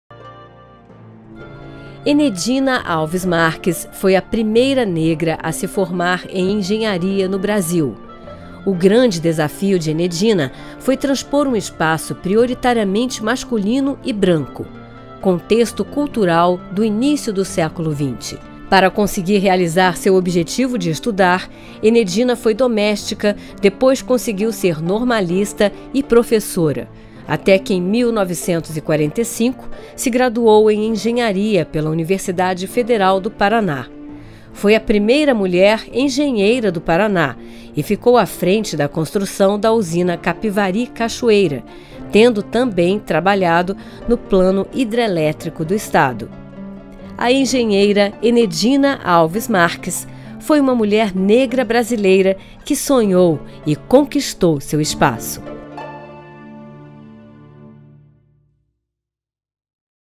Conheça a história de Enedina Alves Marques
INTERPROGRAMAS - 18.07.17: Enedina Alves Marques foi a primeira negra a se formar em engenharia no Brasil. O grande desafio de Enedina foi transpor um espaço prioritariamente masculino e branco, contexto cultural do início do século XX.